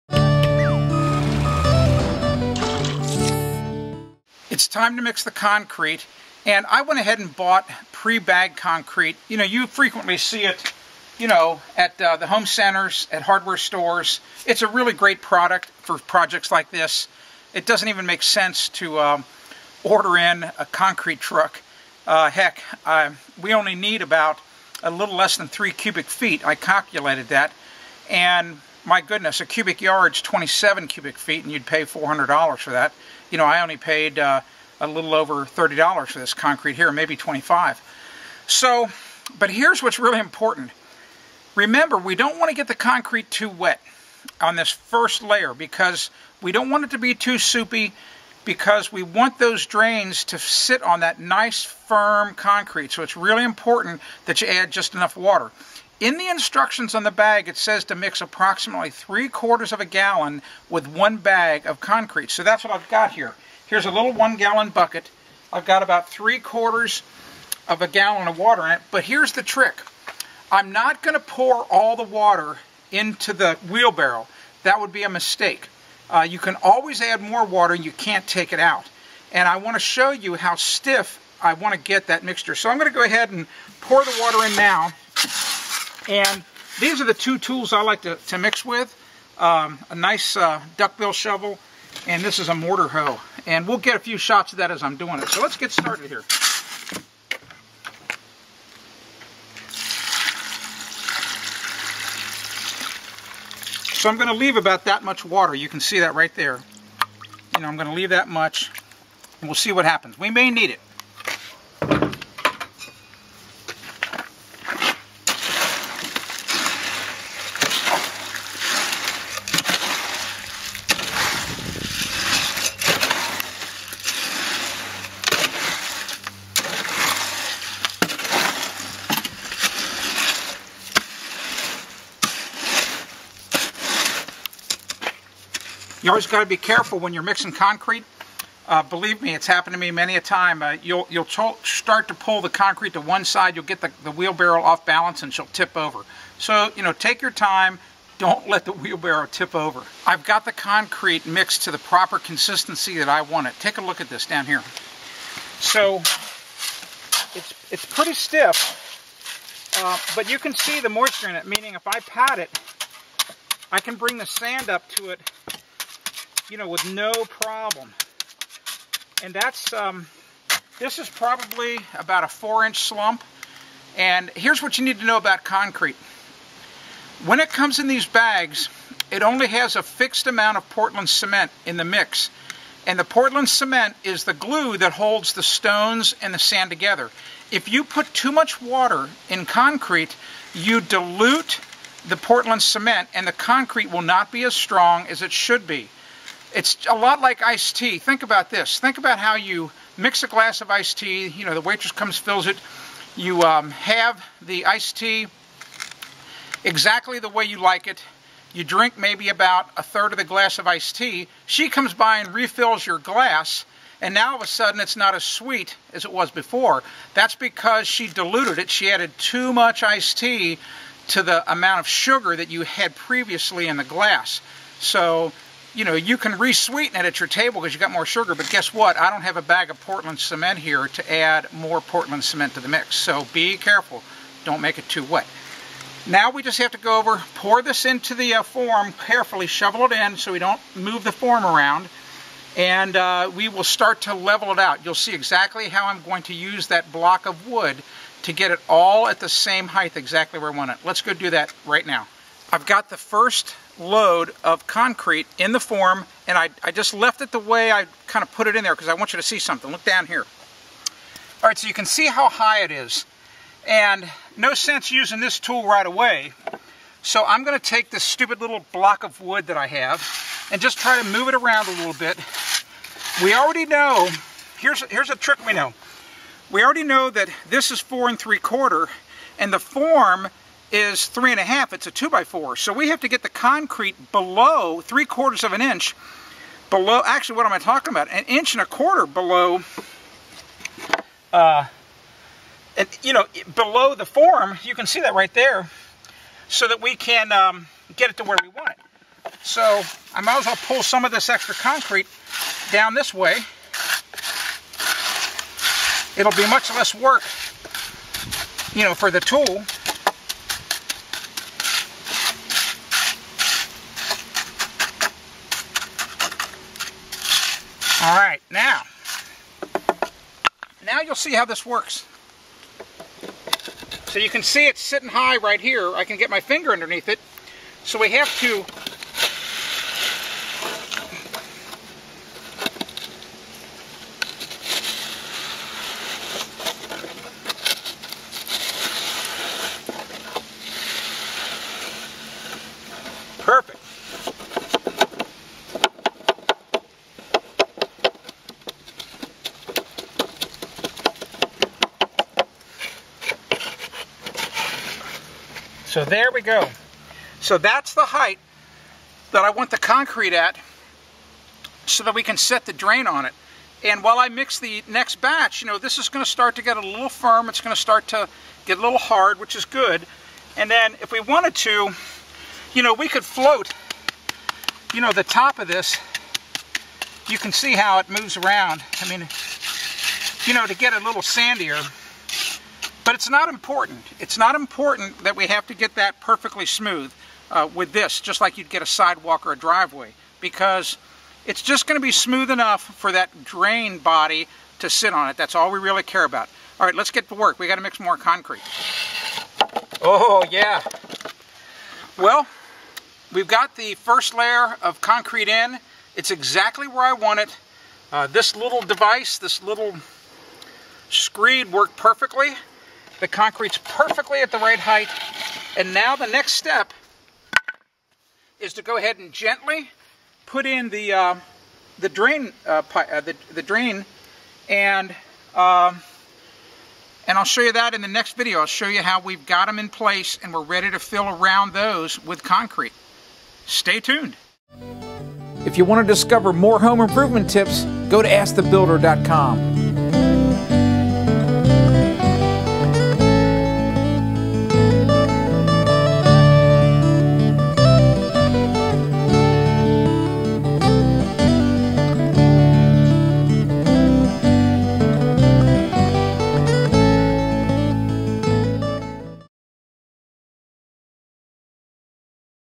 Install a Trench Drain Video 4 of 7
Tim Carter demonstrates how to install a trench drain.